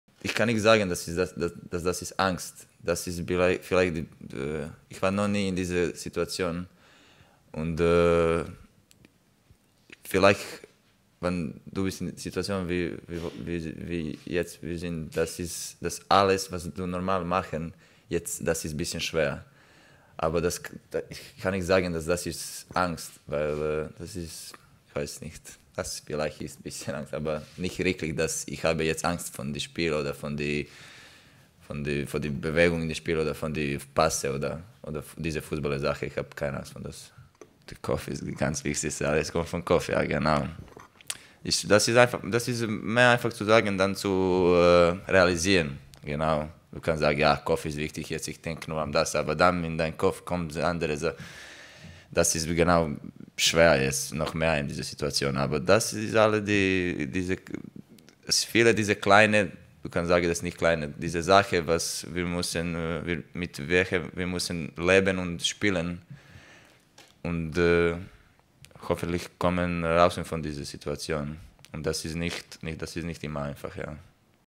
Milan Badelj gesteht: "...vielleicht ein bisschen Angst" | Hamburger SV - Bayer 04 Leverkusen
Der Hamburger SV steckt im Abstiegskampf. Die Lage ist bedrohlich wie nie, der HSV spielt die schlechteste Saison der Vereinsgeschichte. Milan Badelj glaubt aber nicht an den ersten Abstieg des Klubs.